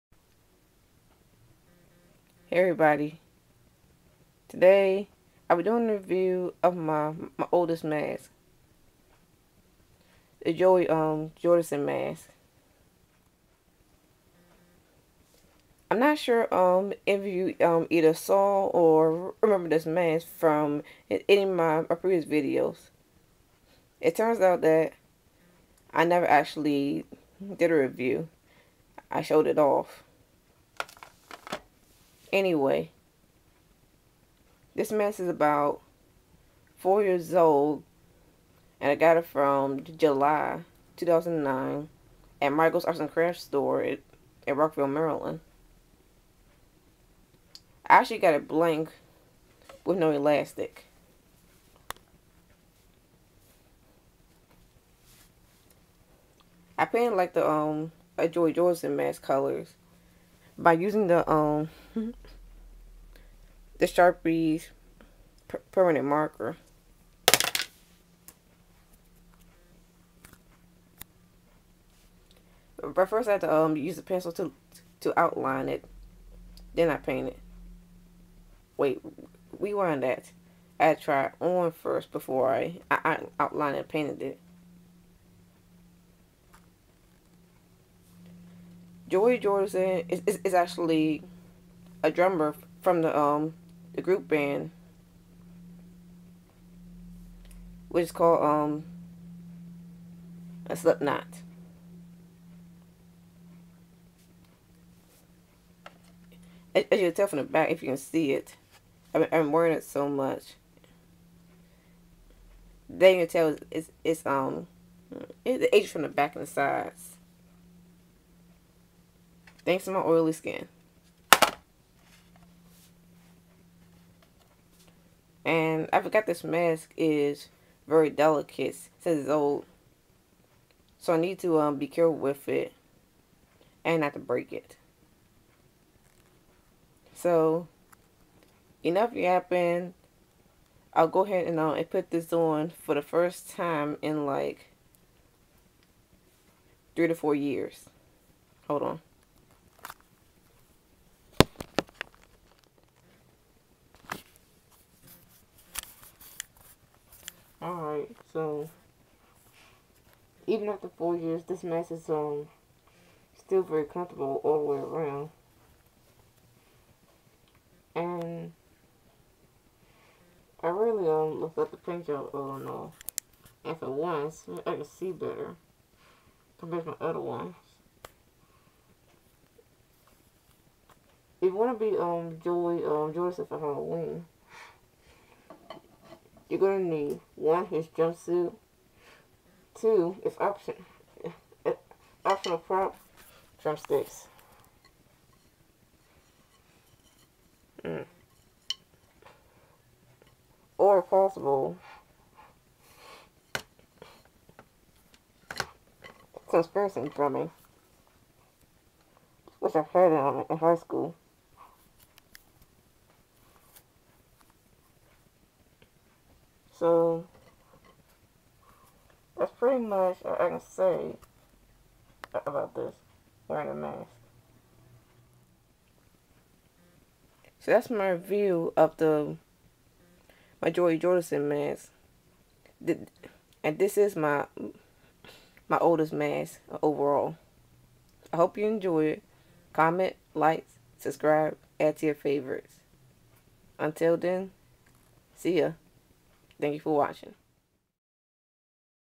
Joey Jordison Mask Review
This is my oldest mask overall so far. I did some showoffs twice with this mask a couple of years ago, but I never got the chance to do the review on it... until now. So here it is!